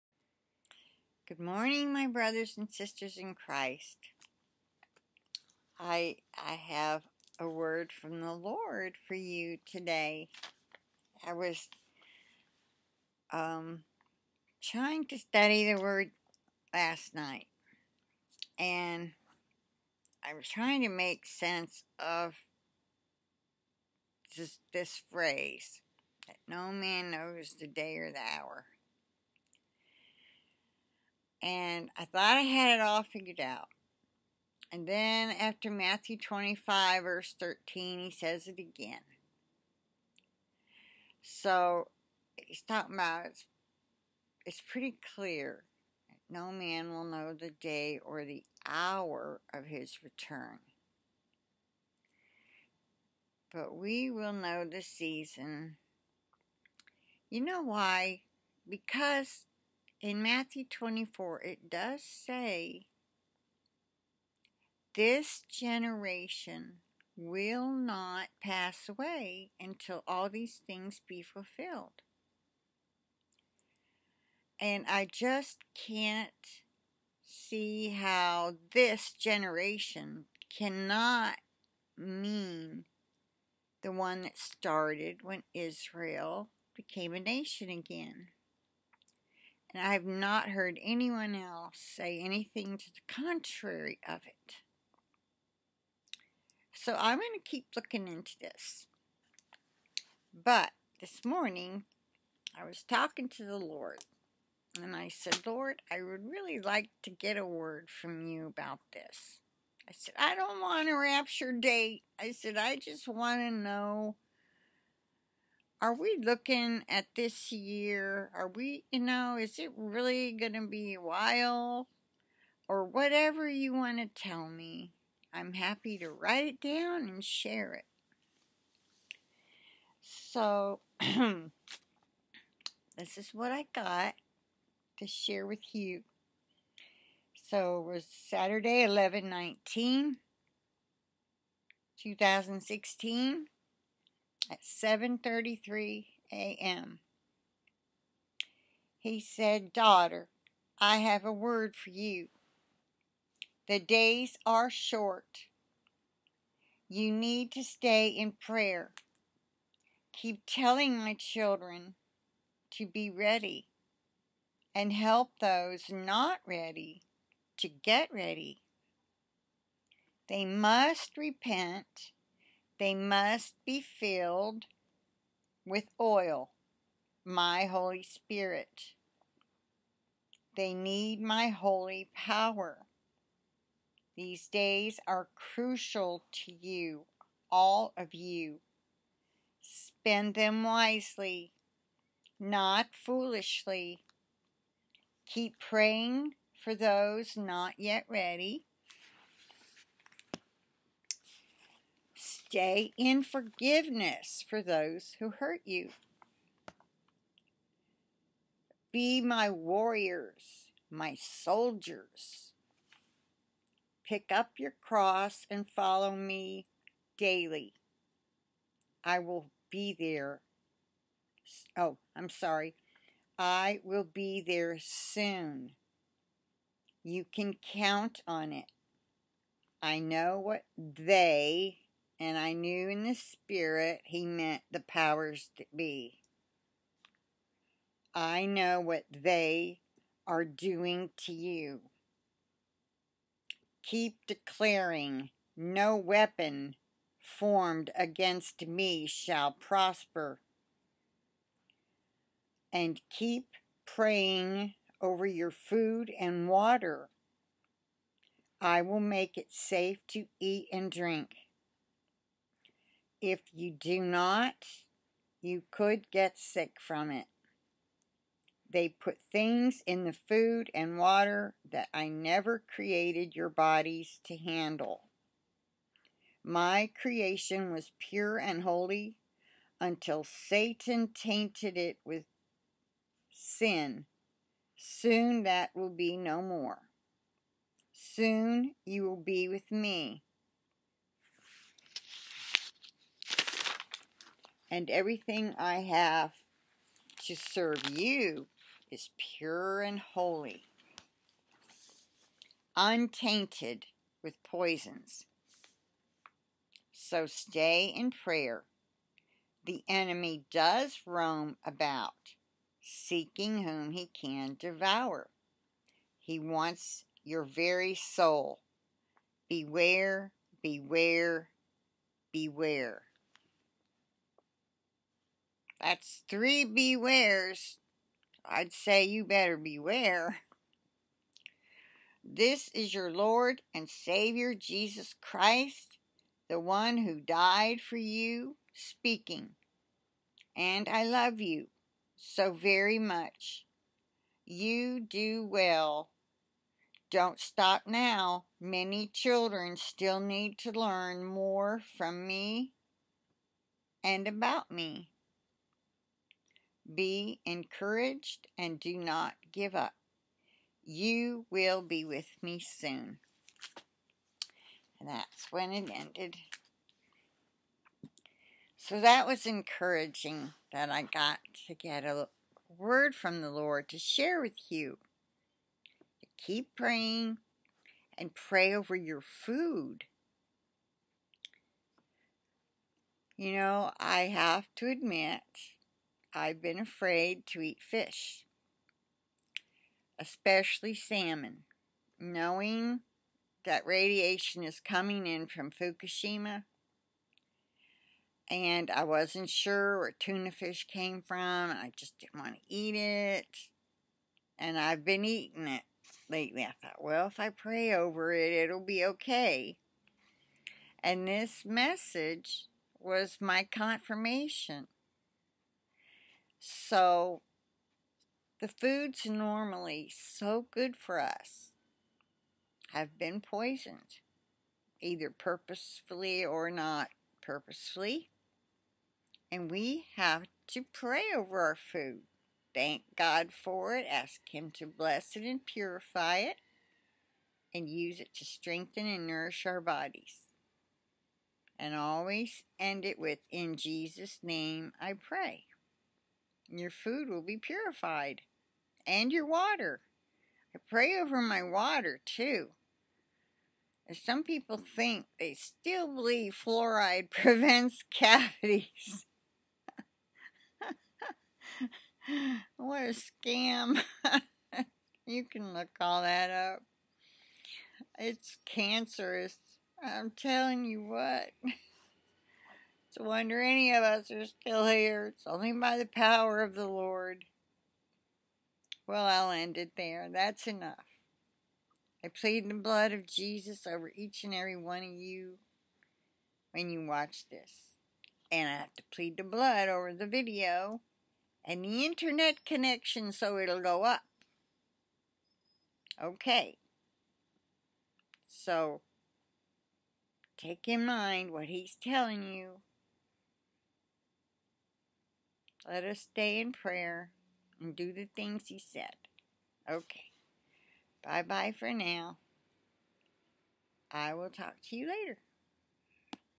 Message from Jesus to STAY IN PRAYER, PRAY OVER FOOD AND WATER!
Scripture references:
 Isaiah 54:17
1 Peter 5:8